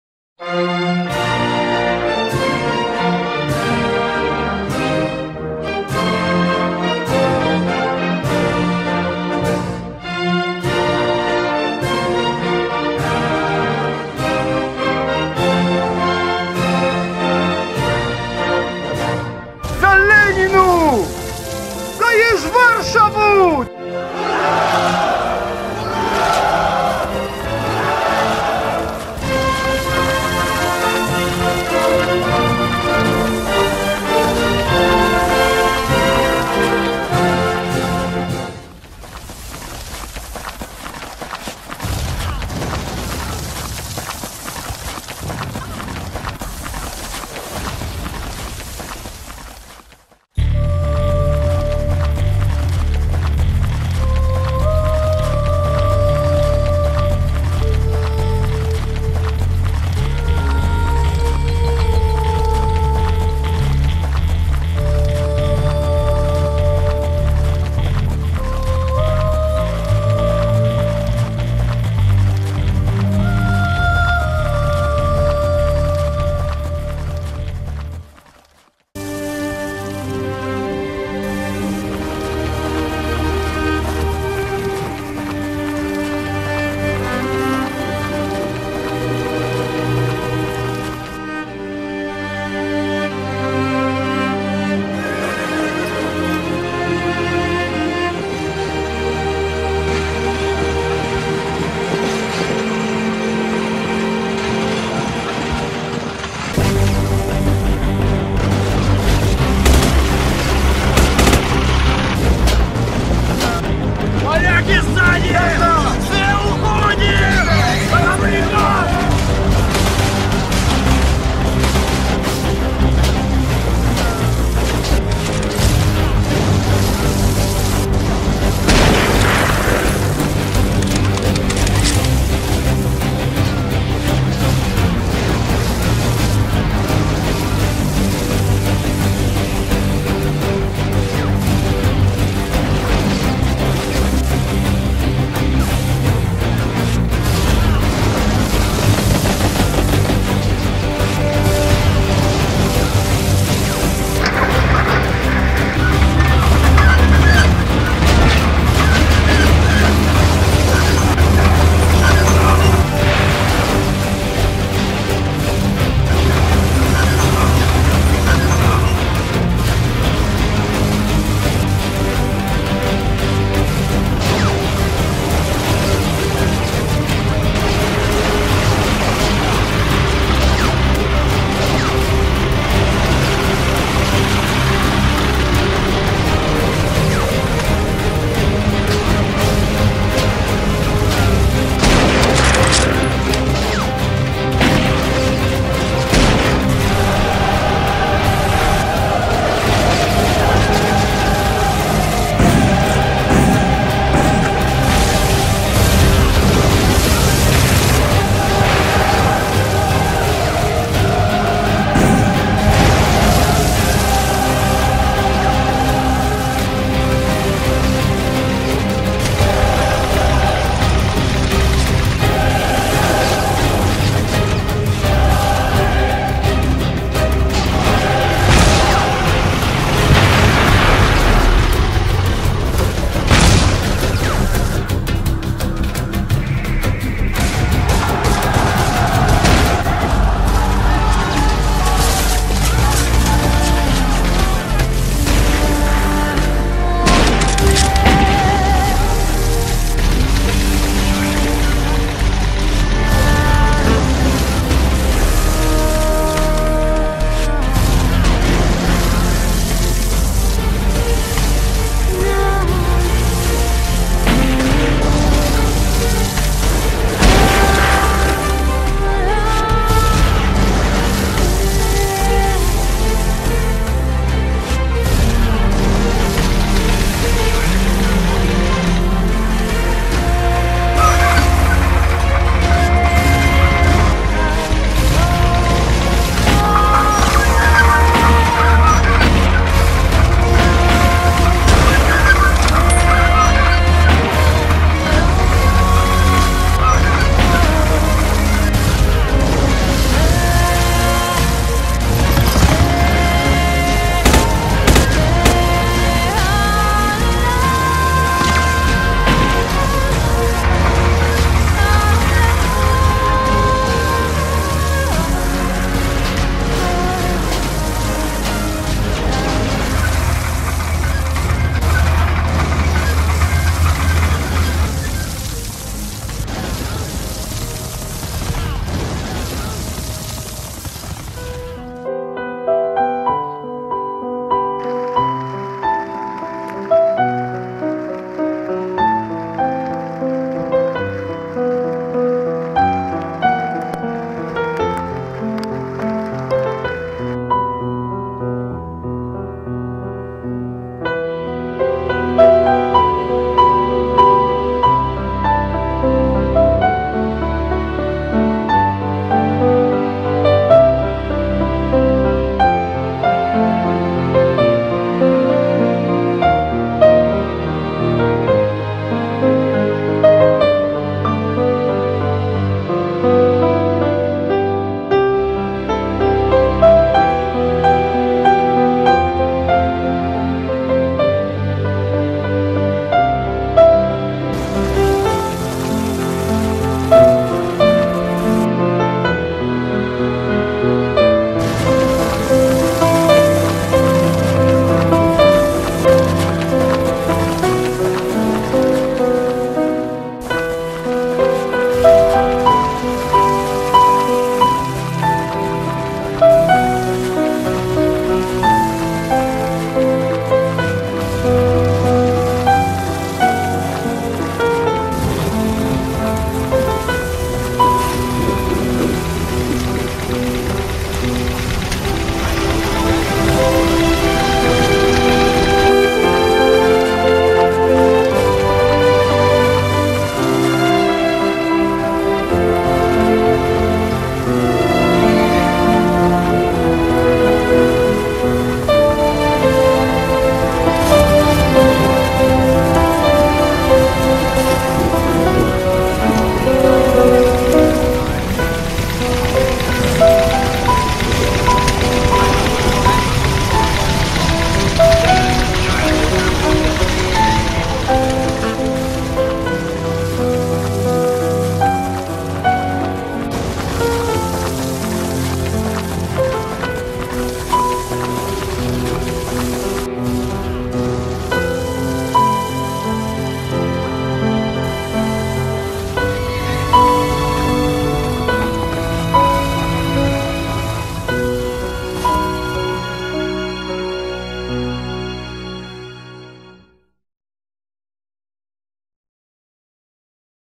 Poľsko-boľševická vojna 1920 - Sarnowa Góra 21.8.2016
Zostrih Petra Ďurišina z voj. ukážky Sarnowa Góra 2016.
Poďakovanie usporiadateľovi ukážky Dariusz Krawczykovi.